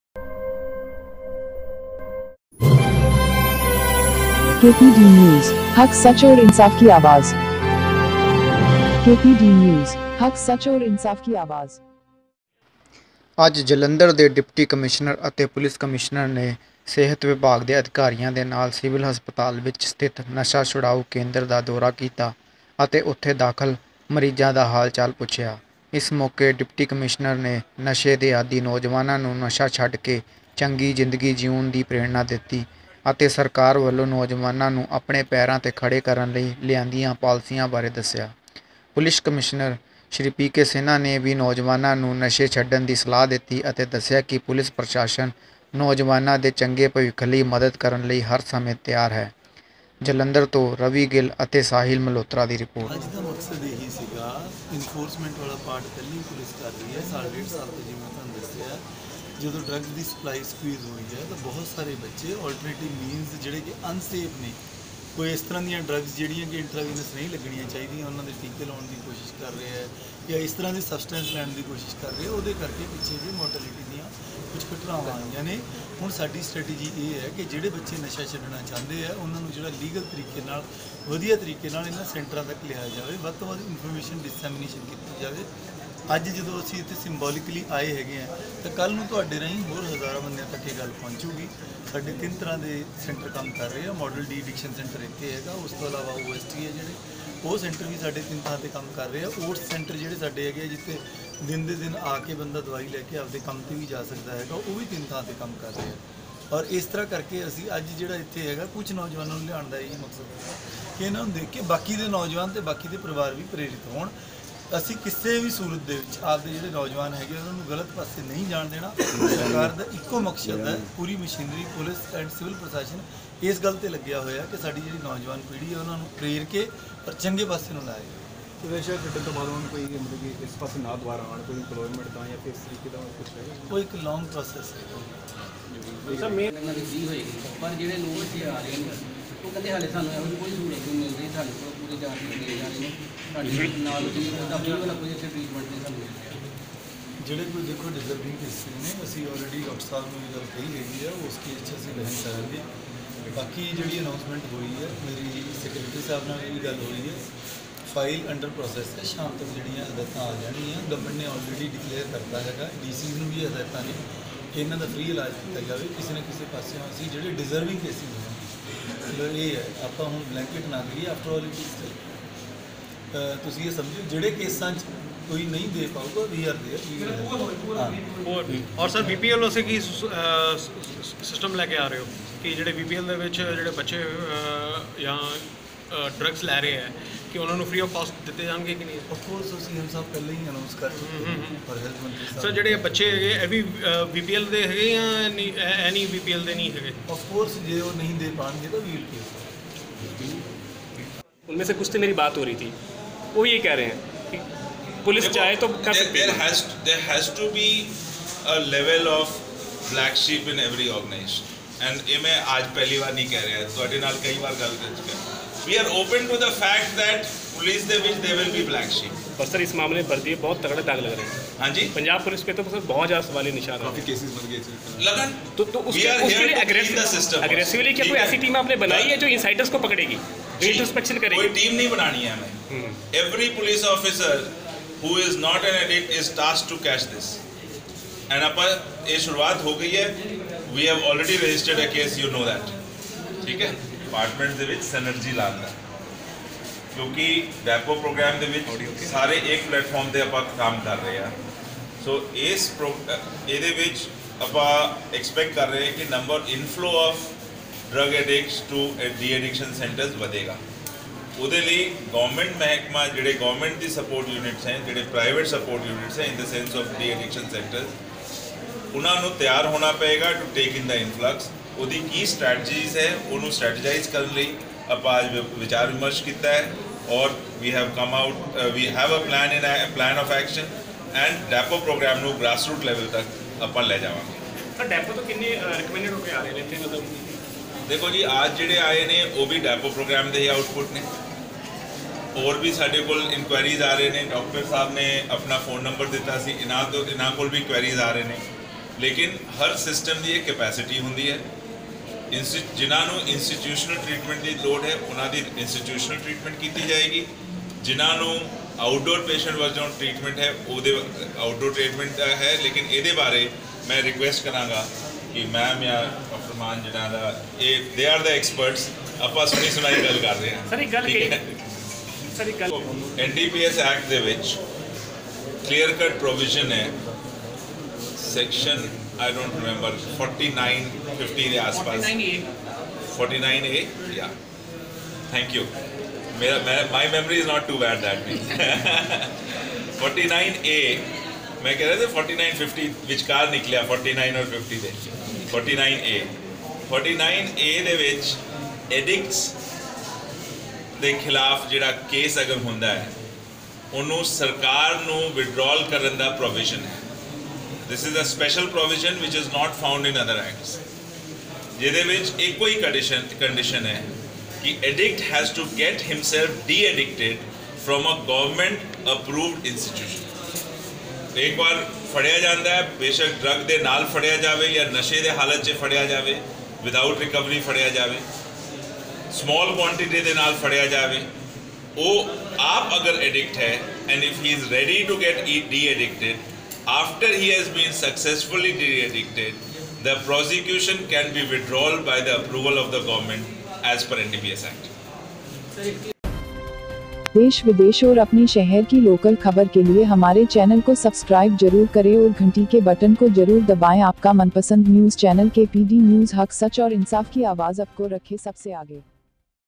ਜਲੰਧਰ ਦੇ ਡਿਪਟੀ ਕਮਿਸ਼ਨਰ ਅਤੇ ਪੁਲਸ ਕਮਿਸ਼ਨਰ ਨੇ ਕੀਤਾ ਨਸ਼ਾ ਛੁਡਾਓ ਕੇਂਦਰ ਦਾ ਦੌਰਾ
ਅੱਜ ਜਲੰਧਰ ਦੇ ਡਿਪਟੀ ਕਮਿਸ਼ਨਰ ਅਤੇ ਪੁਲਸ ਕਮਿਸ਼ਨਰ ਨੇ ਸਿਹਤ ਵਿਭਾਗ ਦੇ ਅਧਿਕਾਰੀਆਂ ਦੇ ਨਾਲ ਸਿਵਲ ਹਸਪਤਾਲ ਵਿਚ ਸਥਿਤ ਨਸ਼ਾ ਛੁਡਾਓ ਕੇਂਦਰ ਦਾ ਦੌਰਾ ਕੀਤਾ ਅਤੇ ਉਥੇ ਦਾਖਲ ਮਰੀਜ਼ਾਂ ਦਾ ਹਾਲਚਾਲ ਪੁੱਛਿਆ। ਇਸ ਮੌਕੇ ਡਿਪਟੀ ਕਮਿਸ਼ਨਰ ਨੇ ਨਸ਼ੇ ਦੇ ਆਦੀ ਨੌਜਵਾਨਾਂ ਨੂੰ ਨਸ਼ਾ ਛੱਡ ਕੇ ਚੰਗੀ ਜ਼ਿੰਦਗੀ ਜਿਉਣ ਦੀ ਪ੍ਰੇਰਣਾ ਦਿੱਤੀ ਅਤੇ ਸਰਕਾਰ ਵੱਲੋਂ ਨੌਜਵਾਨਾਂ ਨੂੰ ਆਪਣੇ ਪੈਰਾਂ 'ਤੇ ਖੜ੍ਹੇ ਕਰਨ ਲਈ ਲਿਆਂਦੀਆਂ ਪਾਲਿਸੀਆਂ ਬਾਰੇ ਦੱਸਿਆ। ਪੁਲਸ ਕਮਿਸ਼ਨਰ ਸ਼੍ਰੀ ਪੀ. ਕੇ. ਸਿਨਹਾ ਨੇ ਵੀ ਨੌਜਵਾਨਾਂ ਨੂੰ ਨਸ਼ੇ ਛੱਡਣ ਦੀ ਸਲਾਹ ਦਿੱਤੀ ਅਤੇ ਦੱਸਿਆ ਕਿ ਪੁਲਸ ਪ੍ਰਸ਼ਾਸਨ ਨੌਜਵਾਨਾਂ ਦੇ ਚੰਗੇਰੇ ਭਵਿੱਖ ਲਈ ਮਦਦ ਕਰਨ ਲਈ ਹਰ ਸਮੇਂ ਹਾਜ਼ਰ ਹੈ।
ਜਲੰਧਰ ਤੋਂ ਰਵੀ ਗਿੱਲ ਅਤੇ ਸਾਹਿਲ ਮਲਹੋਤਰਾ ਦੀ ਰਿਪੋਰਟ